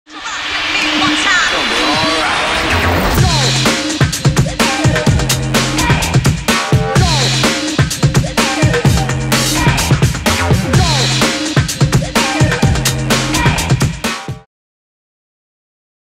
Jaguar I-PACE | Відчуйте справжні емоції
Танцюйте, насолоджуйтесь, самовиражайтесь та ловіть момент разом із неймовірним Jaguar I-PACE. Готові до справжніх емоцій? Ми з @andrey_stateva точно готові вразити вас.

video by @andrey_stateva